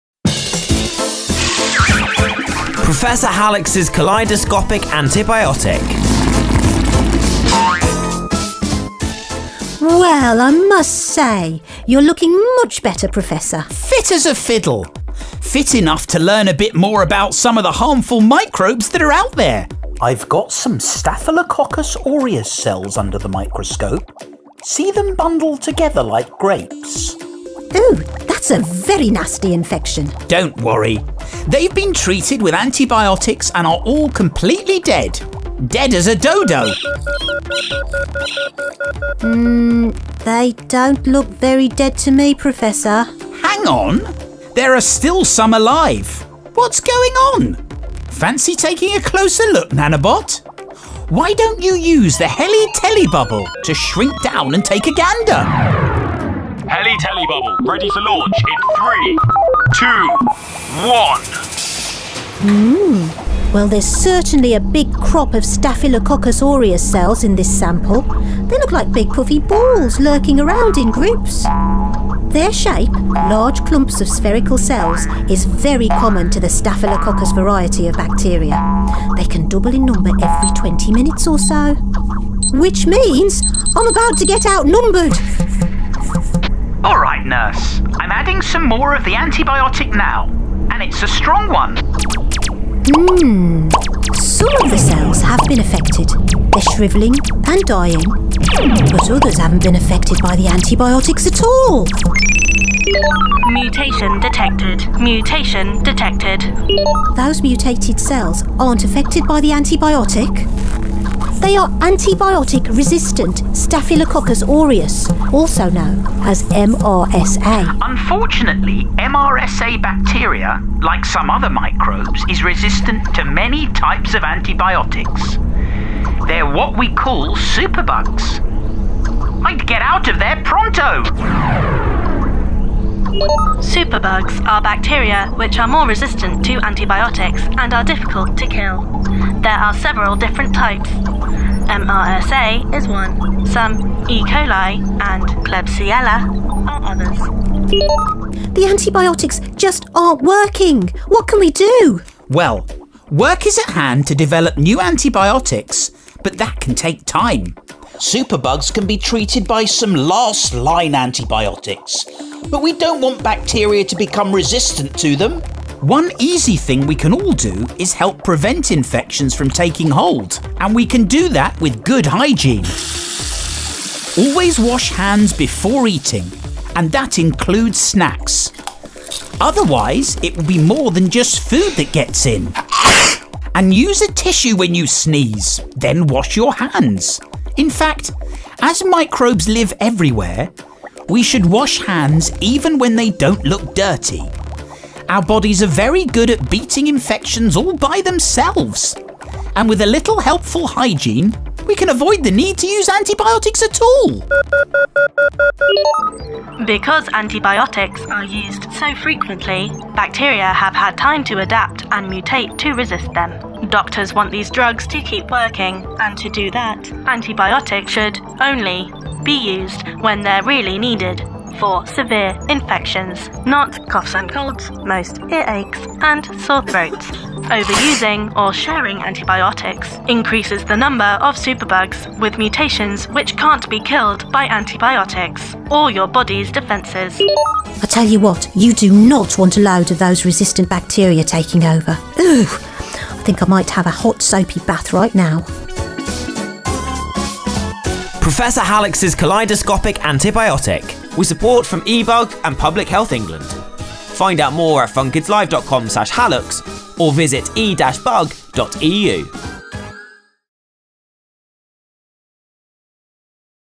Professor Hallux’s Antibiotics: Episode 4: Survival of the fittest
In this episode Hallux notices the bacteria under his microscope has resisted treatment with antibiotics: they are MRSA, a very common superbug. Work is at hand to develop new antibiotics but that can take time.